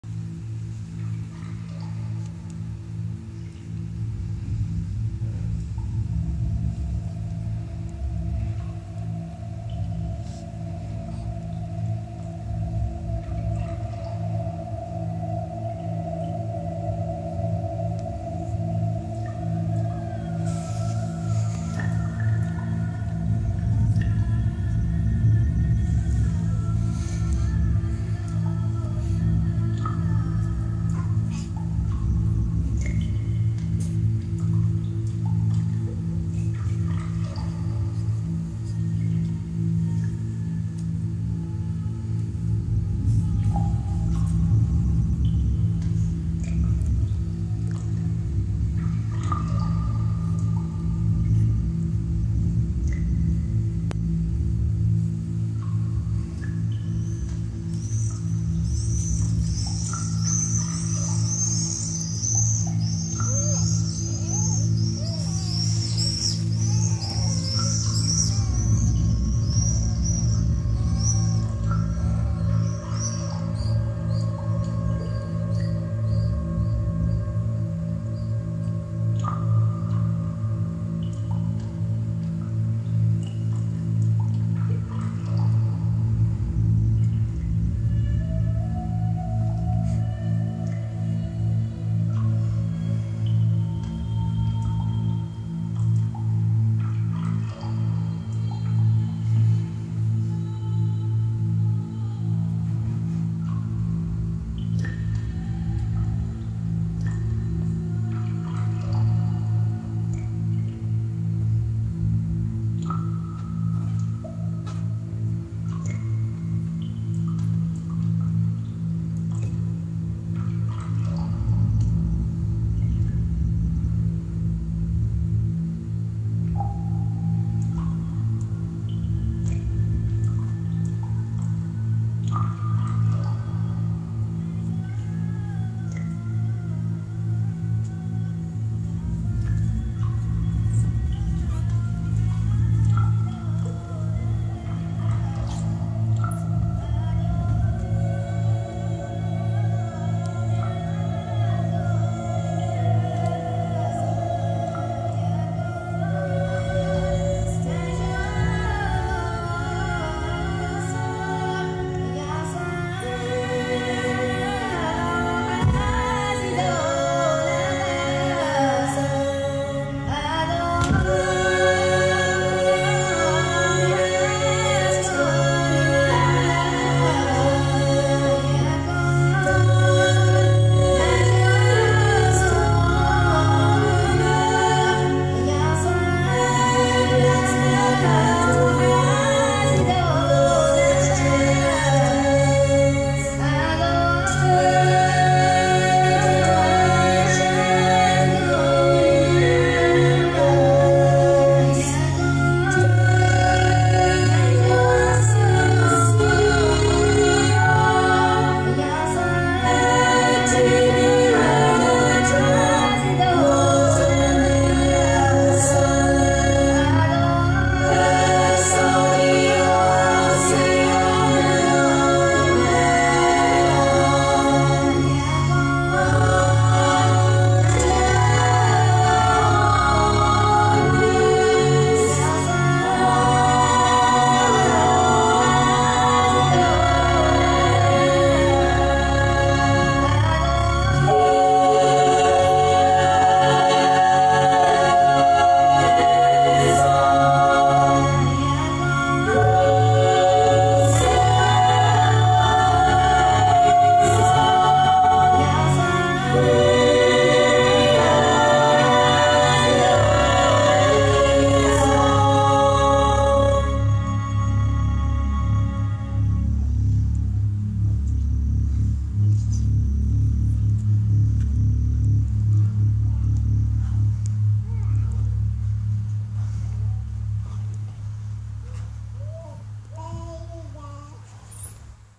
Johan Hillgren - Solo
Work in progress presentation of a solo at Balettakademien Stockholm.